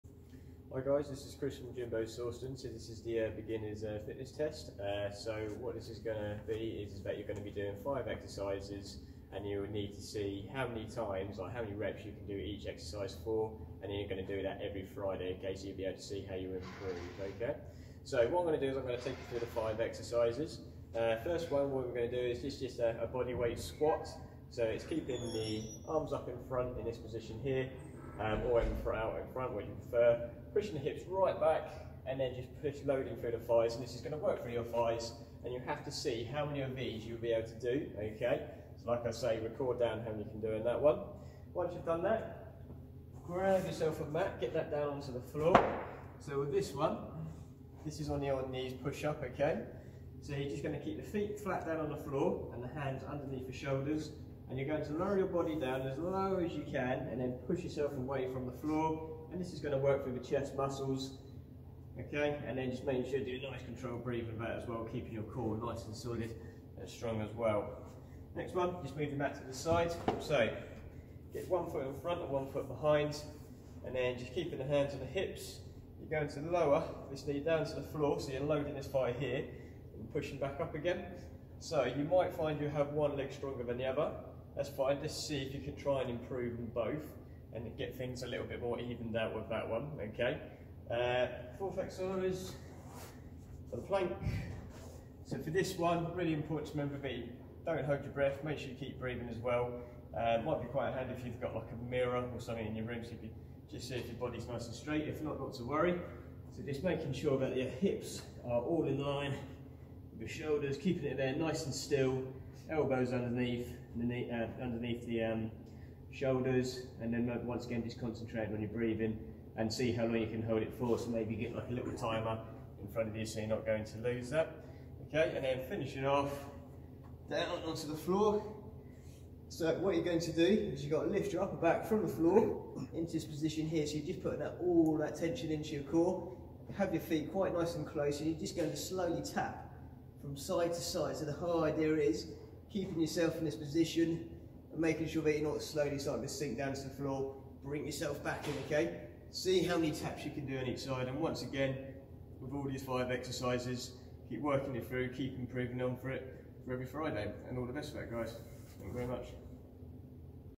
Beginners Fitness Test
Instructions:
Perform each exercise for maximum reps / time (until form failure).
Rest for exactly 60 seconds in between each exercise, no less no more.
Write down your score and compare to last week.